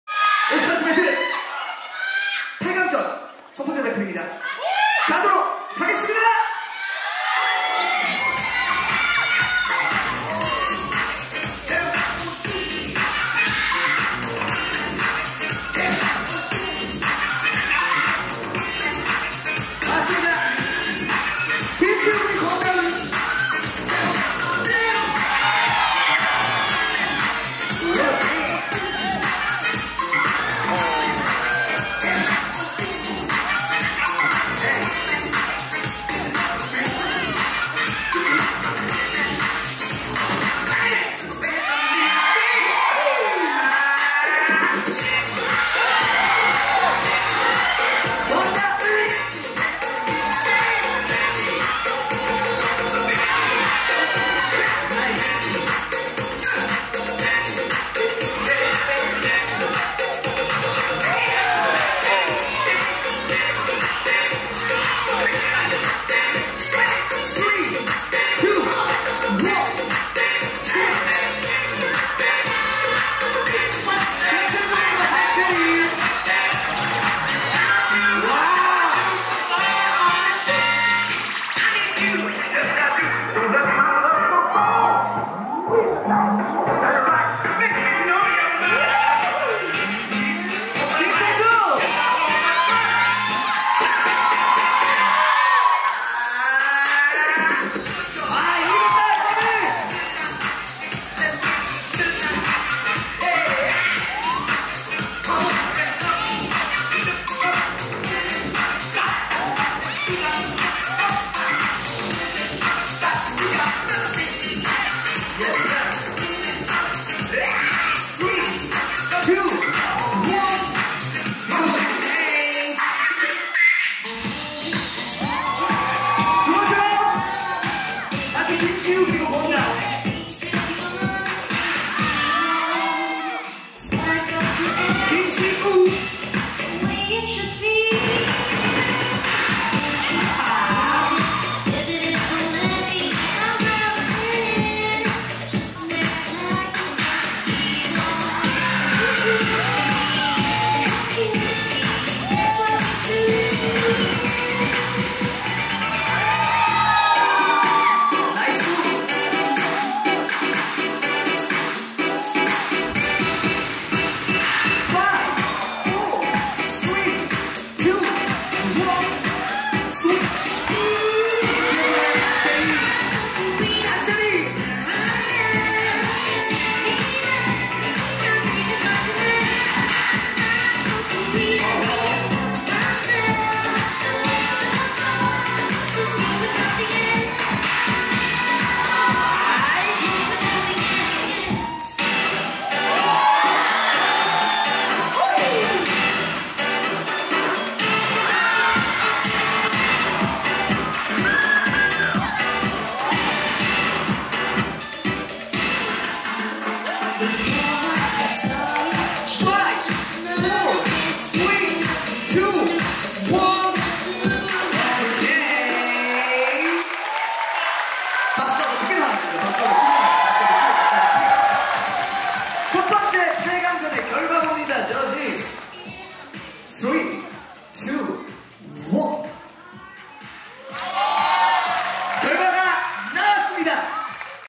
[올스타즈썸머콘서트] 2019 올스타즈 썸머콘서트 배틀 / 8강 - 1 / 세종올스타즈댄스학원
올스타즈 썸머 콘써트 8강 1번째 배틀

김시우,권다은 vs 김태조,한채린